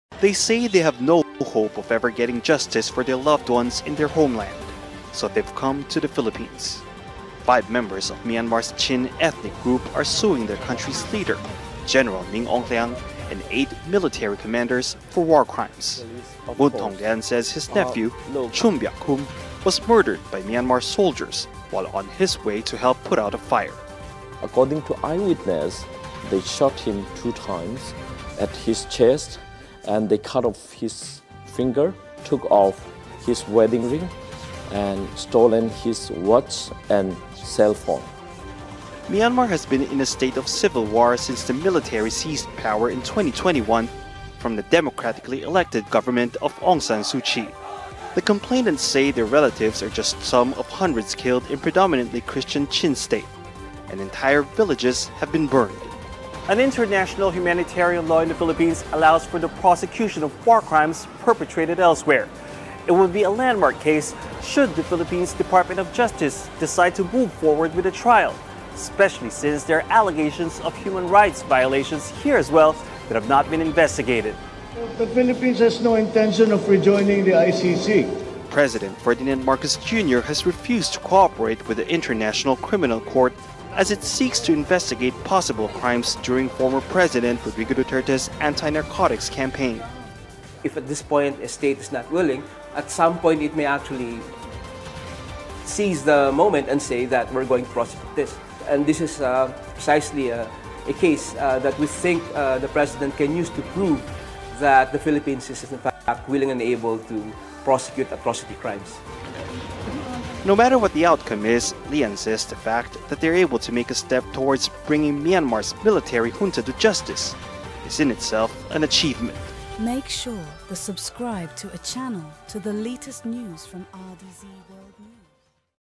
Myanmar war crimes allegations: Junta taken to court in the Philippines | RDZ World News
Five Myanmar nationals are taking the ruling military junta to court in the Philippines.
They're asking the country's Justice Department to look into war crimes committed against the Chin ethnic minority group.
But the Philippine government itself has been criticized for failing to prosecute human rights abuses, during former President Rodrigo Duterte’s war on drugs.